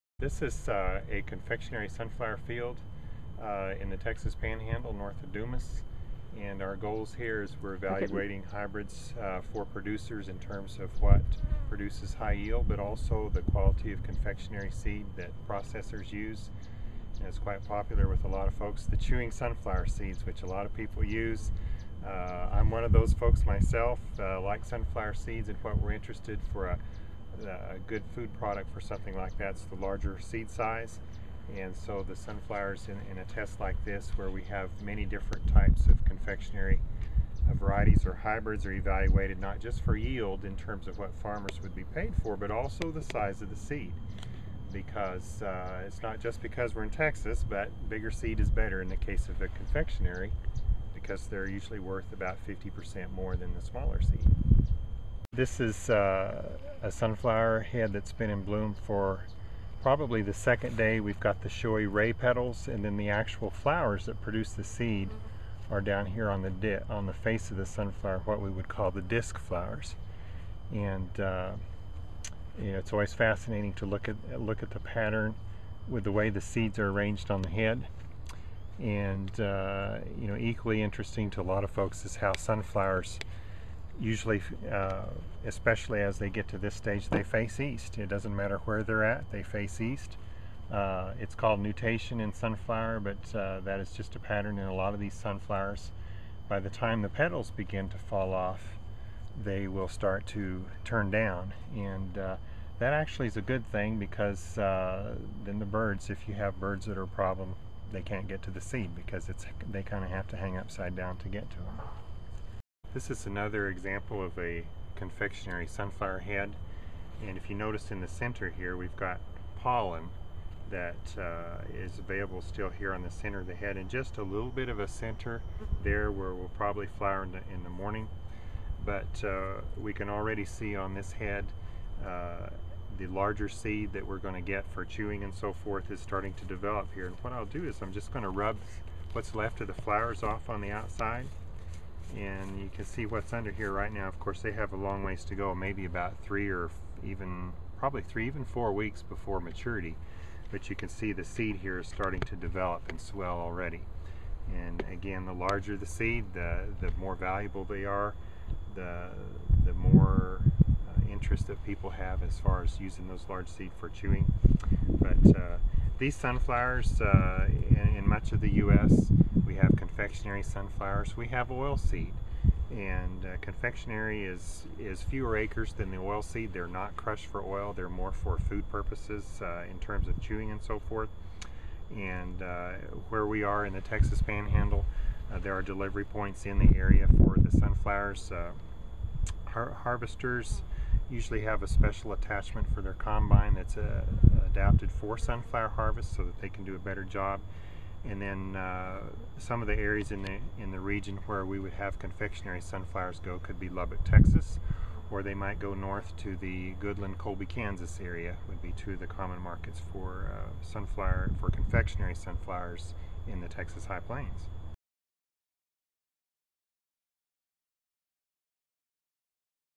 Texas sunflower production on the High Plains
Calvin Trostle, agronomist with the Texas AgriLife Extension Service in Lubbock, discusses both confectionary and oilseed sunflowers grown in the High Plains of Texas.